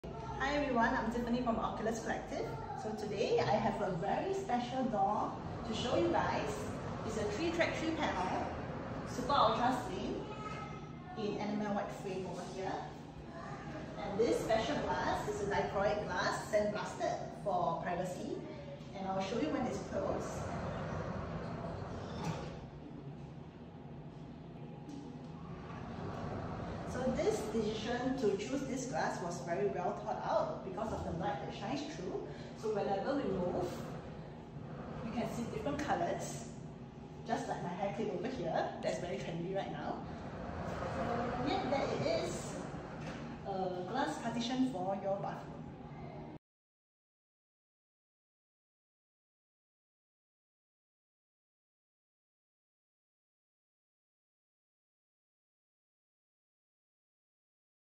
Oculus Collective - Aluminium sliding glassdoors in Dichroic Fluted Frosted Laminated glass
Oculus Collective - Aluminium sliding glassdoors in Dichroic Fluted Frosted Laminated glass (Enamel white frame)

Whatsapp our door experts and let us build your perfect doors!